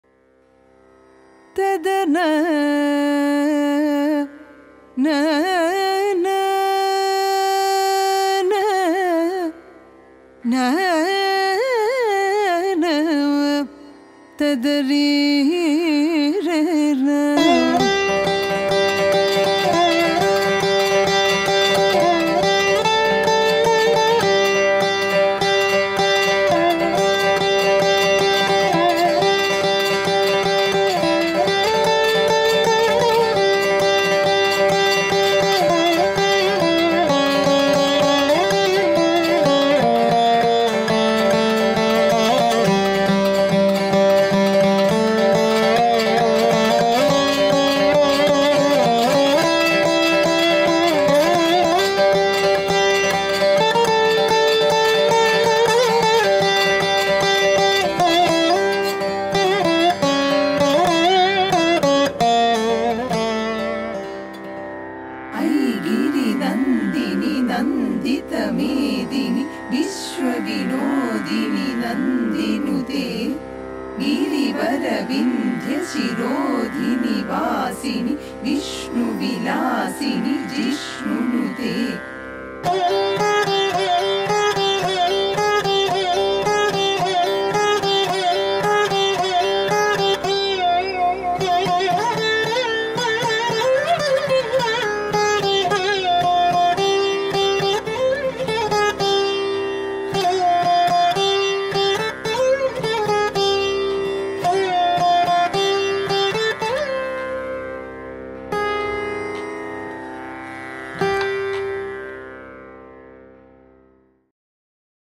Bhaskar's Arts Academy Bhairavi trailer
Bhairavi -- the absolute, unquenchable hunger that propels all life. The power of the Divine Feminine.